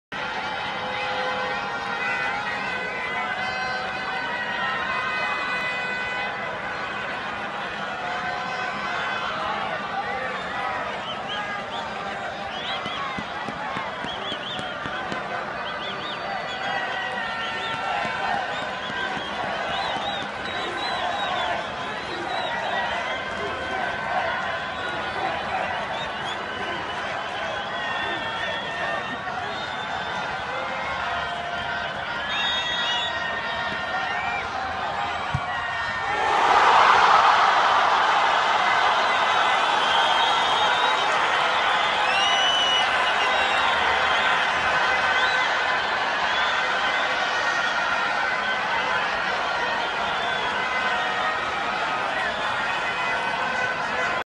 Ecuador 1 Brasil 2
Gol del descuento para la mini TRI en el sudamericano 2017